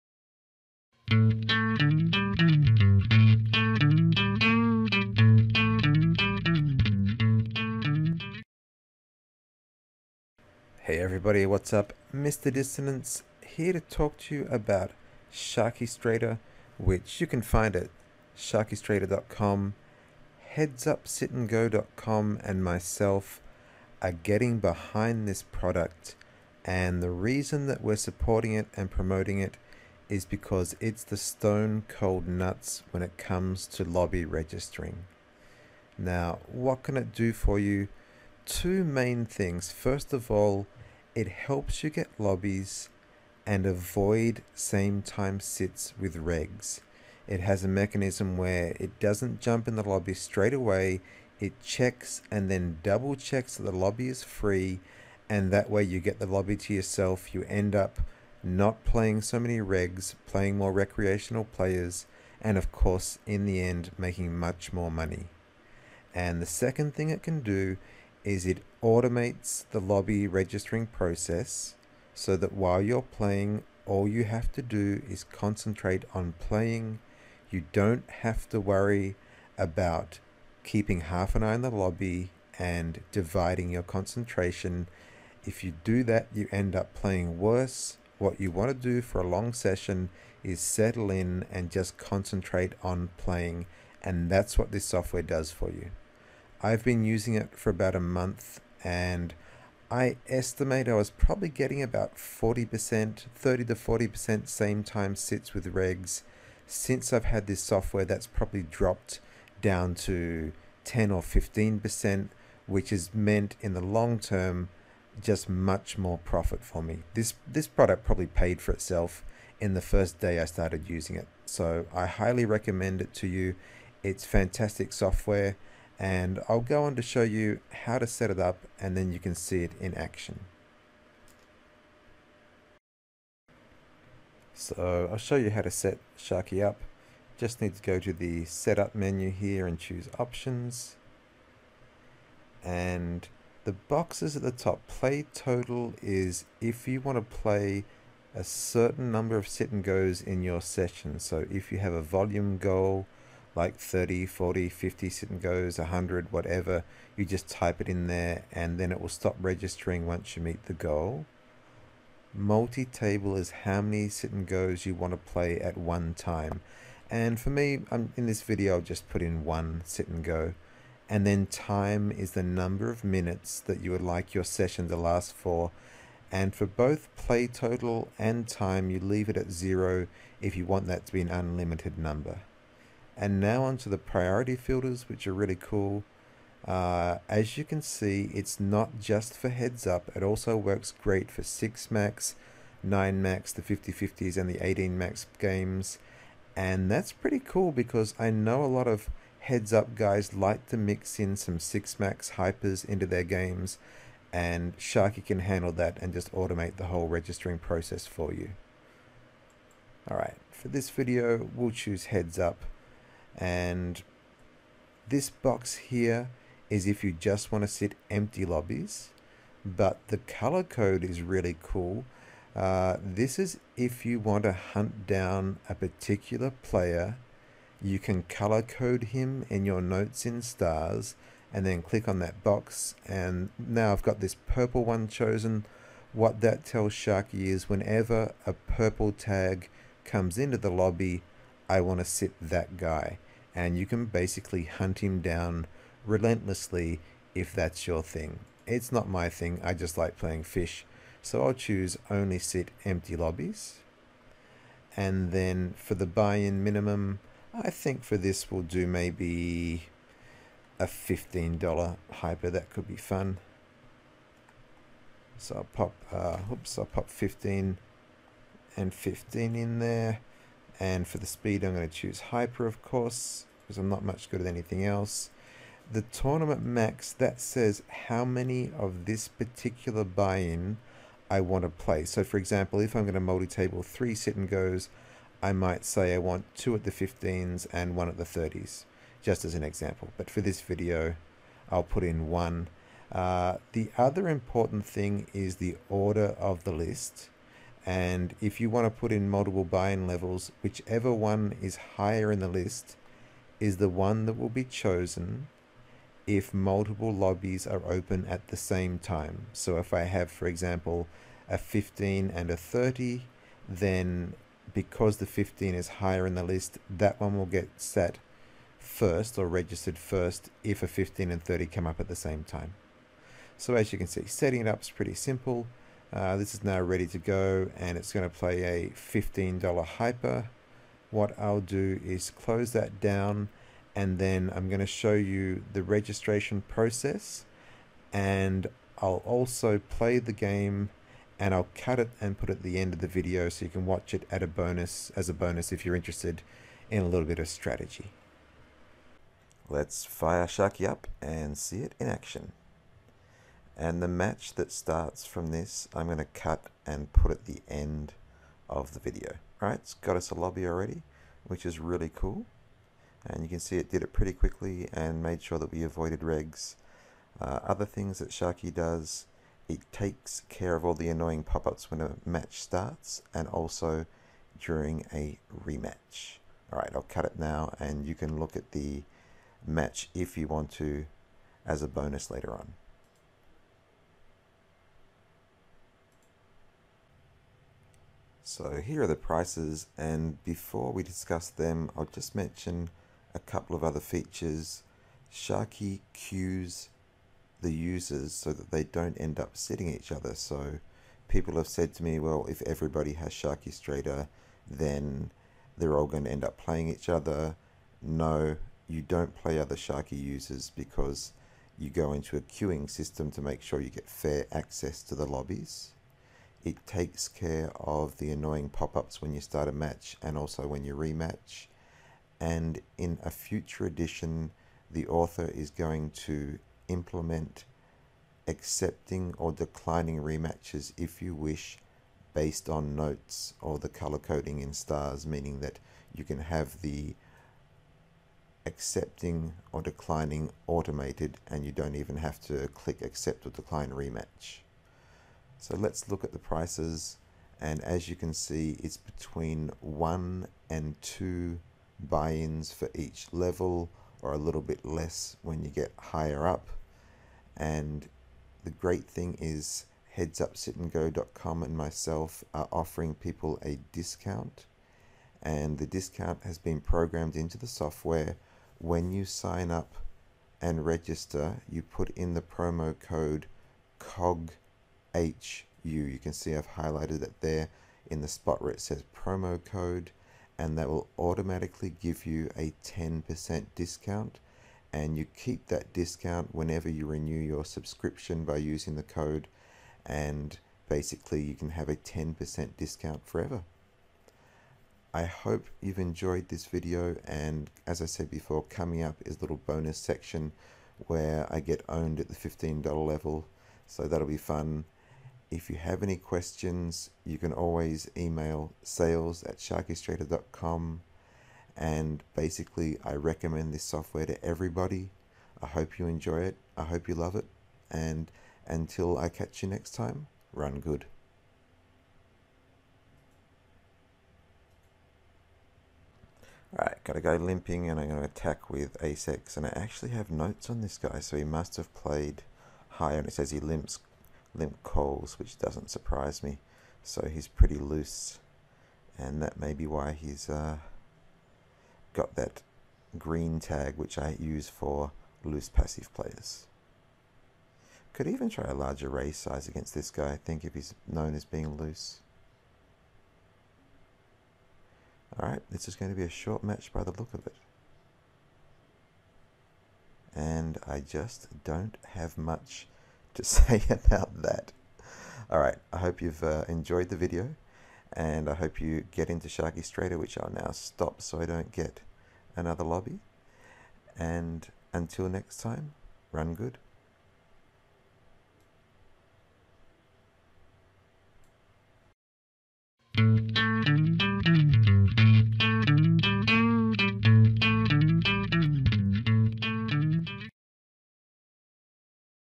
Cog Dissonance Reviews Poker Program SharkyrStrator - Fast Lobby Registration
Cog Dissonance of reviews the SharkyStrator program. He talks about how to save money when purchasing the program, the benefits of using it for regular heads up poker players and other details about the popular poker software.